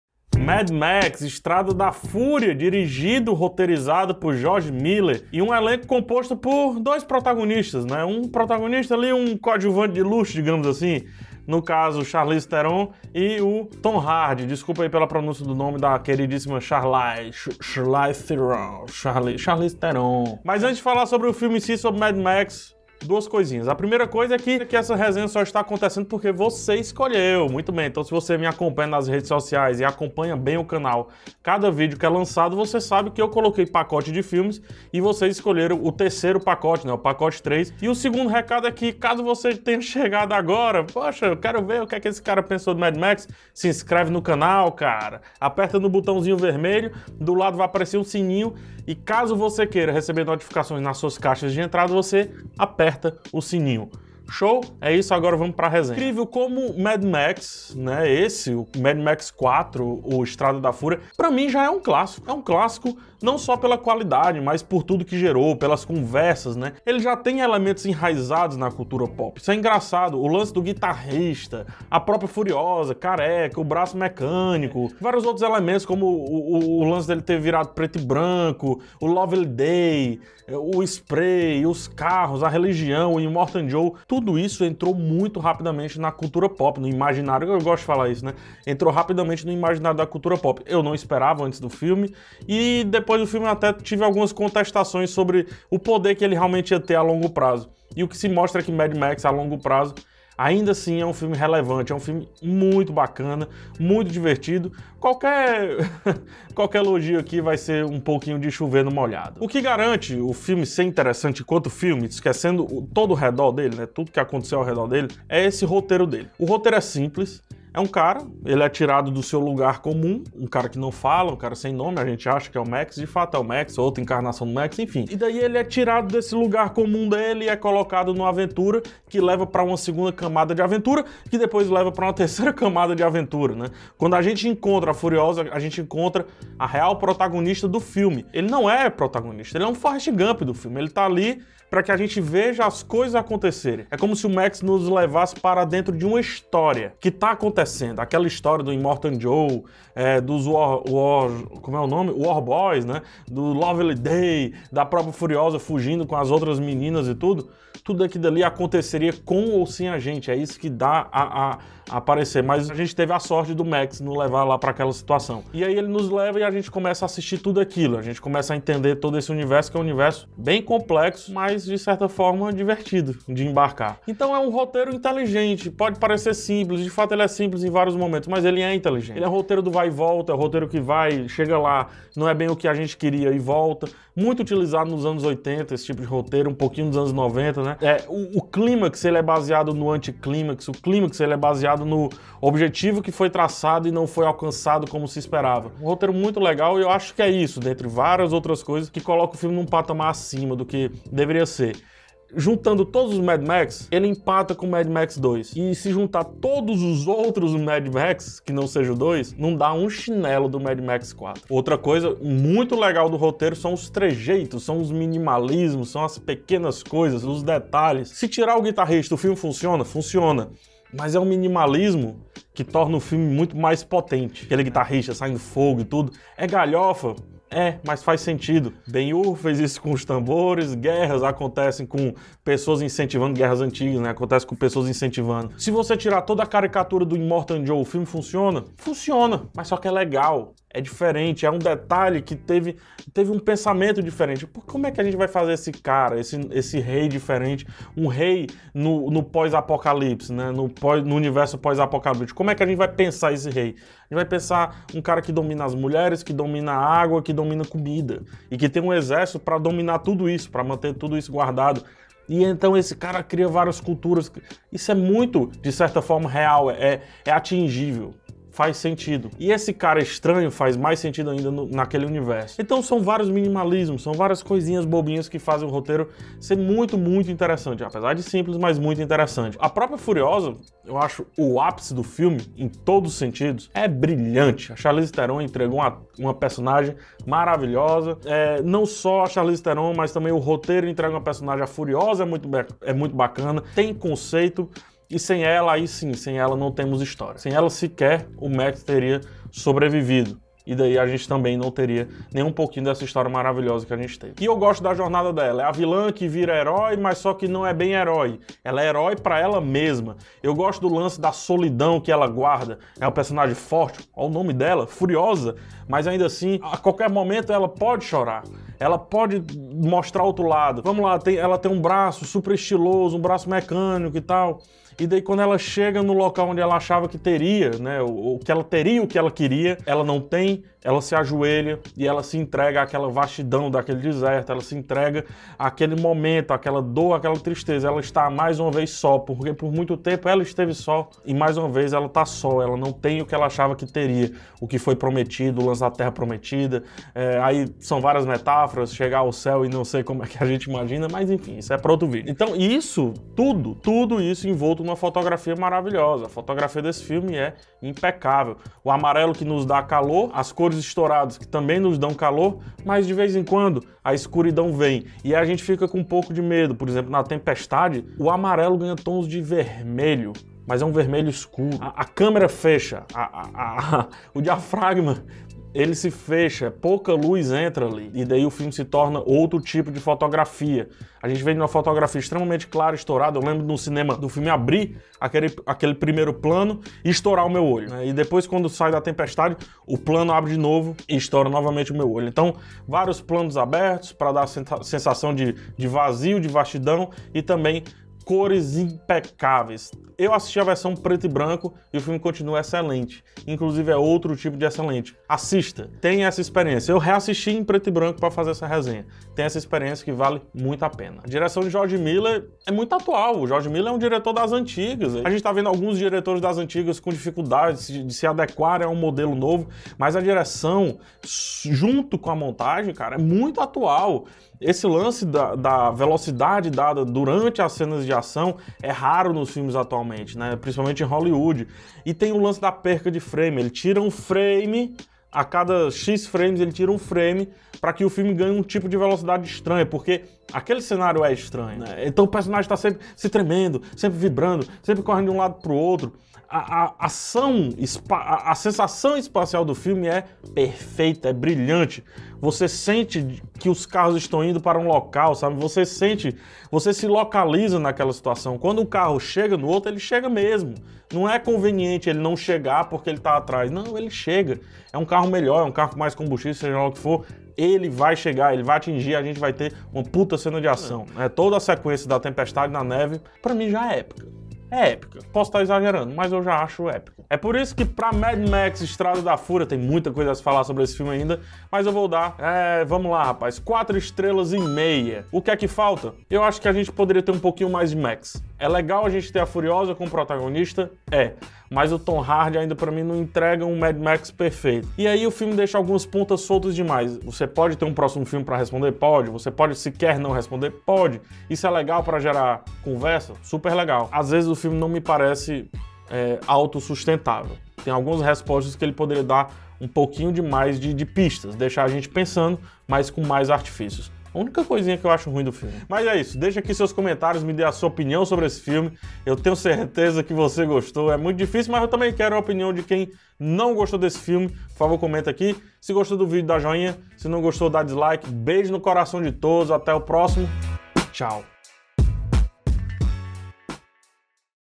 MAD MAX - Estrada da Fúria (2015) | Crítica
Vocês escolheram o pacote dos novos clássicos e por isso está aí a resenha de MAD MAX - Estrada da Fúria. Agora é assistir e comentar logo aqui em baixo. Partiu? :)